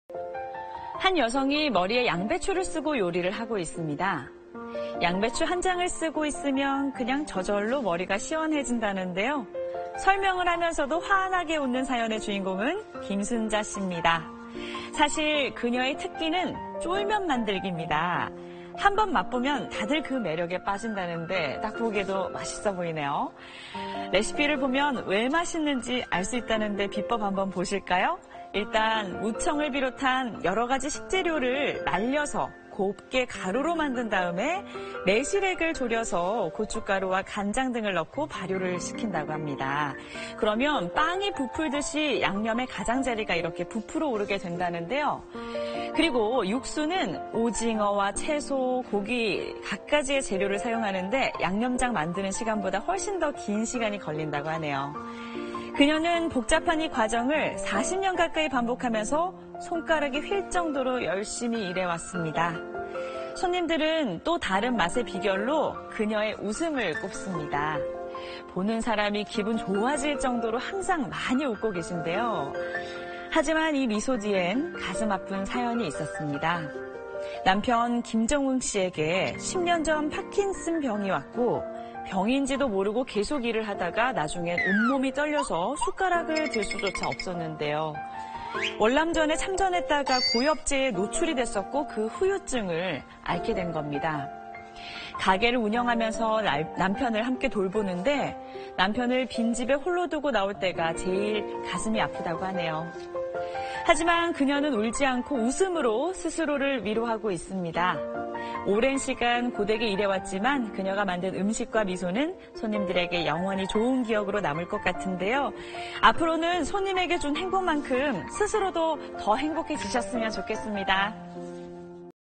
양배추 쓴 쫄면 아주머니…웃음 속에 숨은 뭉클한 사연
양배Chu~♡ 머리 위에 Chu~♡